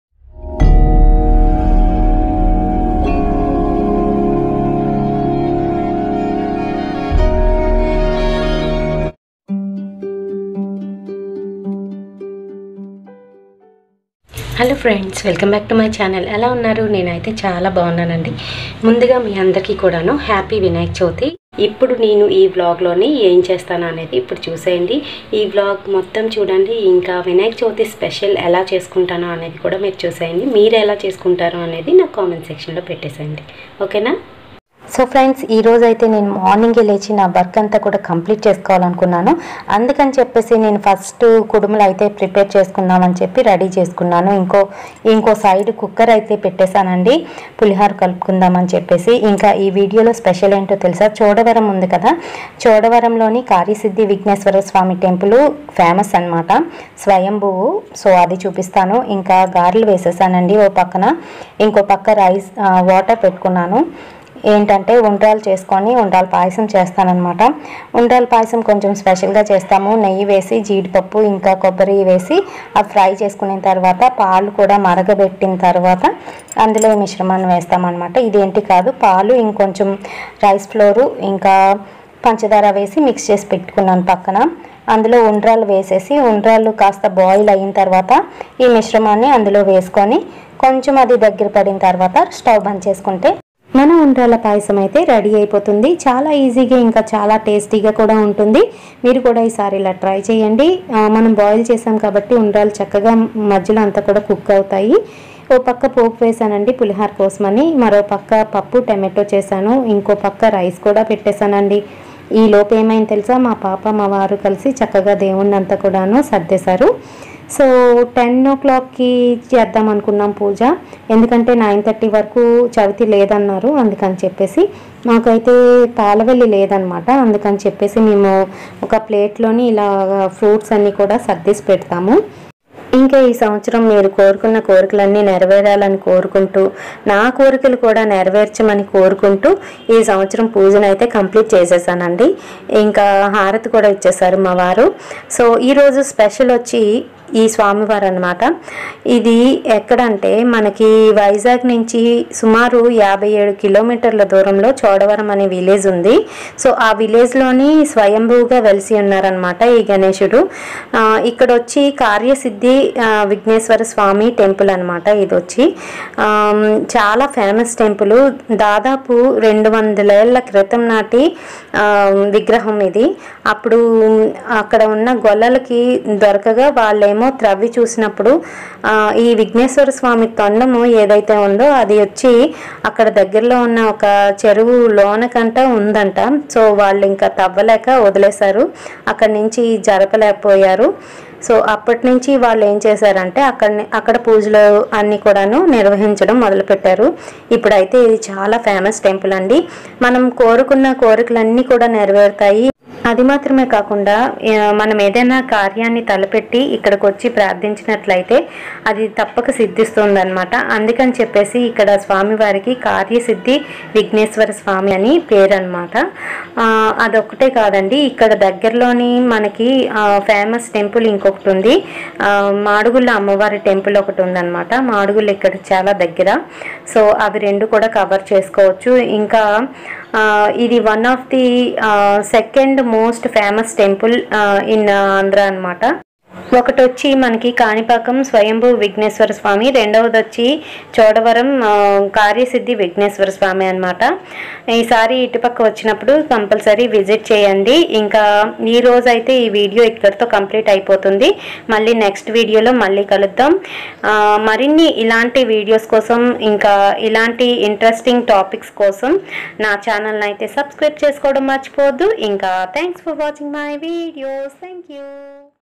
Ganesh chaturthi special//karya siddhi vinayak temple swayambhu//Anjani's channel//telugu
Hi friends,
Today ganesh chaturthi special vlog and the second most famous temple in Andhra Shri karya siddhi vinayak temple @ chodavaram